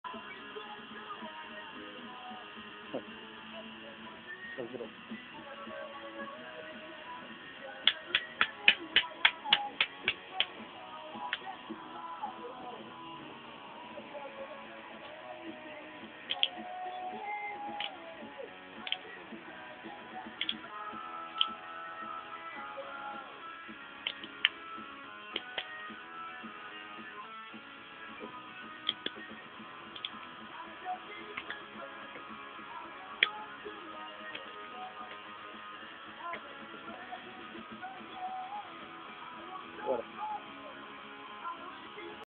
百円ショップ の 自転車 鍵破壊
百円ショップの自転車の鍵は叩けば30秒程で開く見たいです。